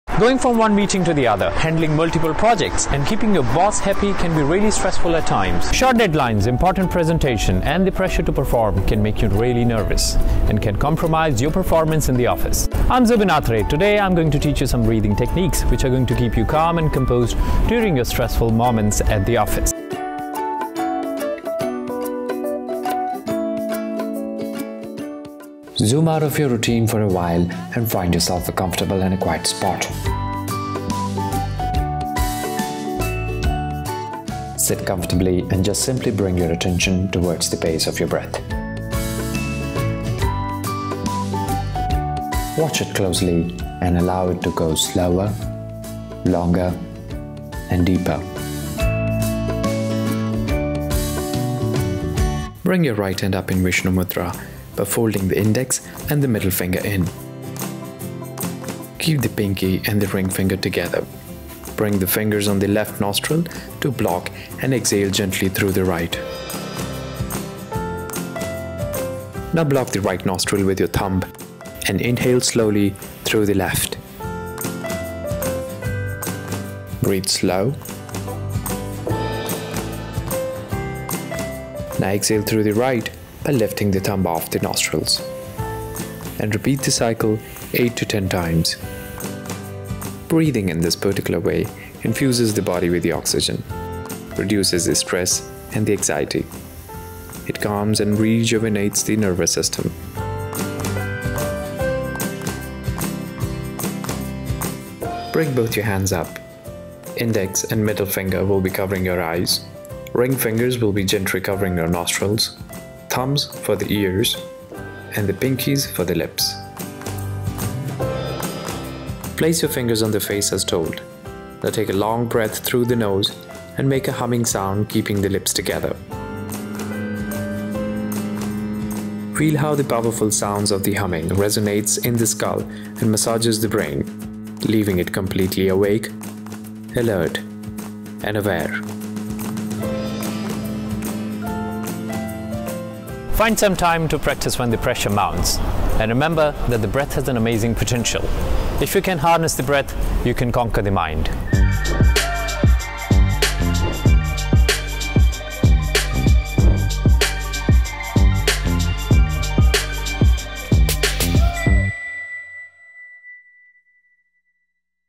Yoga With Zubin Ep 3: Work Stress Got You Down? Try these Easy Breathing Exercises | Quint Fit
Stress at work can weigh you down and impact your performance. Yoga guru Zubin Atre in his 3rd episode of the series explains easy breathing exercises you can do to calm yourself and focus on work.